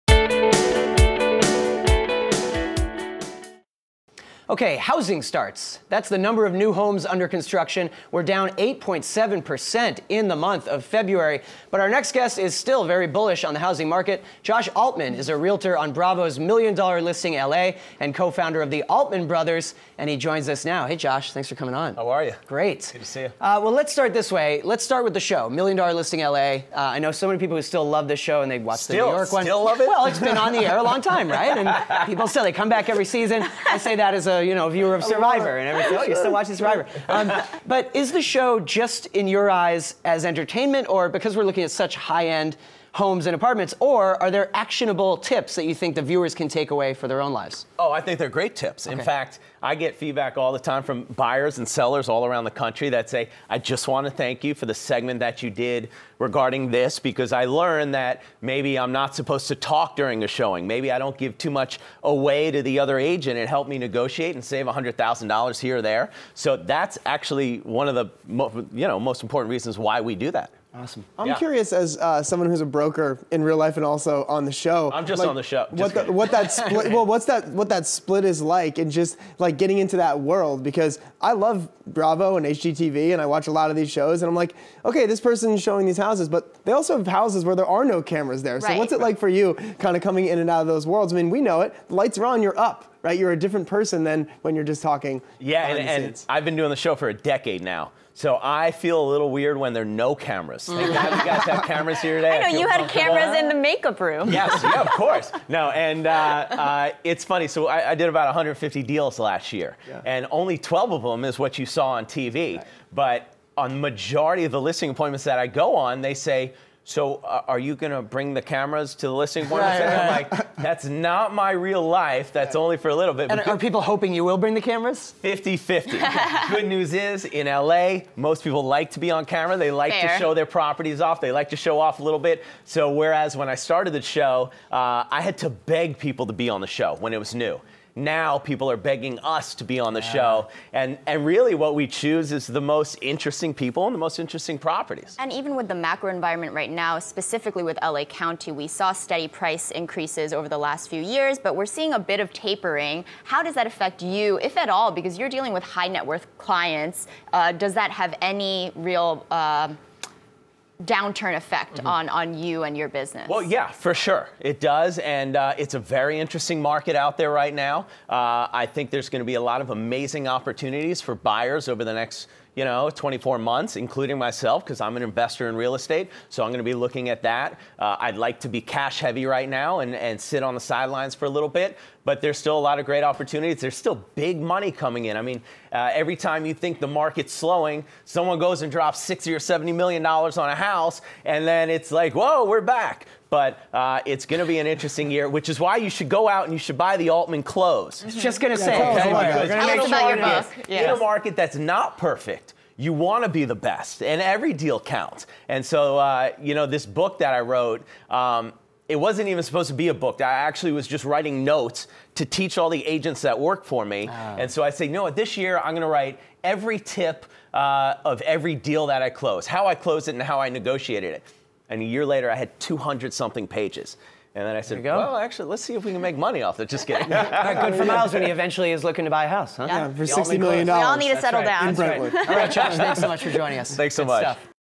"There is going to be amazing opportunity in real estate," Josh Altman
Realtor on Bravo's "Million Dollar Listing L.A., discusses trends in the real estate market and  his new book, "The Altman Close."